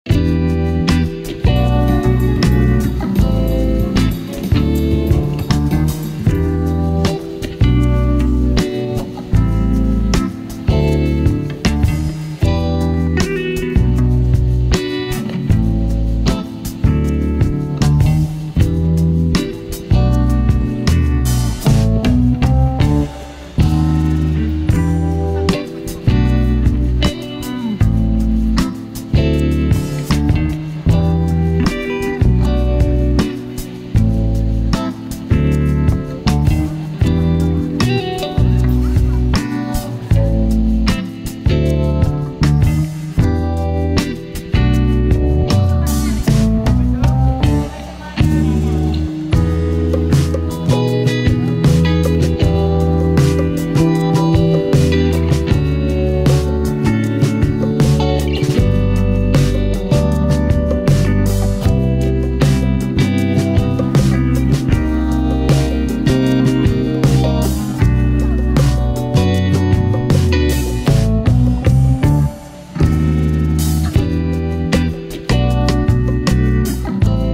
Visit Malaysia - Kota Kinabalu
Gaya Street Market every Sunday (morning till noon).